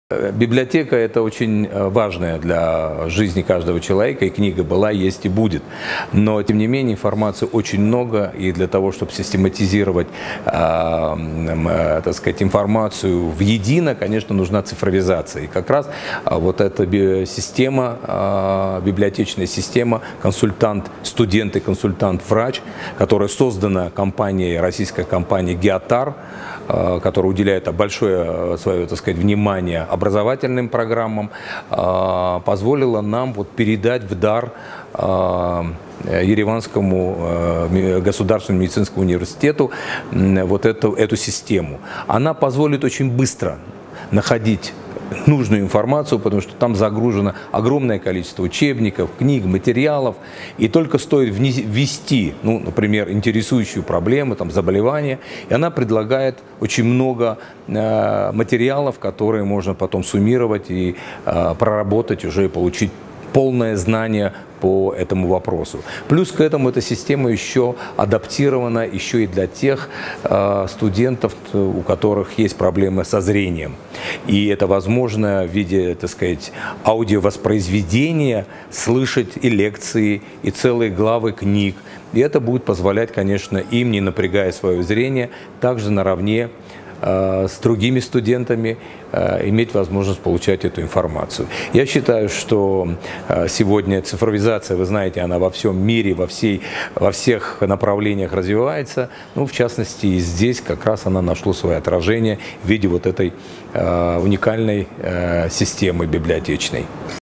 Ապագա բժիշկները ստացան գրադարանային եզակի համակարգից օգտվելու հնարավորություն
🔊«Գրադարանը շատ կարևոր է յուրաքանչյուր մարդու կյանքում․ գիրքը եղել է, կա և կլինի»,-այս համոզմանն է ԵՊԲՀ պատվավոր դոկտոր, Ռուսաստանի գիտությունների ակադեմիայի թղթակից անդամ, ՌԴ գիտության վաստակավոր գործիչ, ակադեմիկոս Արմաիս Կամալովը, որն իր նախագահությամբ գործող «Ժառանգություն և առաջընթաց» հիմնադրամի և գործընկեր՝ ՌԴ ամենամեծ բժշկական հրատարակչության «ԳԷՕՏԱՐ-Մեդիա»-ի հիմնադիր, ռուս գիտնական Գուզել Ուլումբեկովայի անունից ԵՊԲՀ-ին նվիրաբերեց գրադարանային թվային համակարգից օգտվելու հնարավորություն: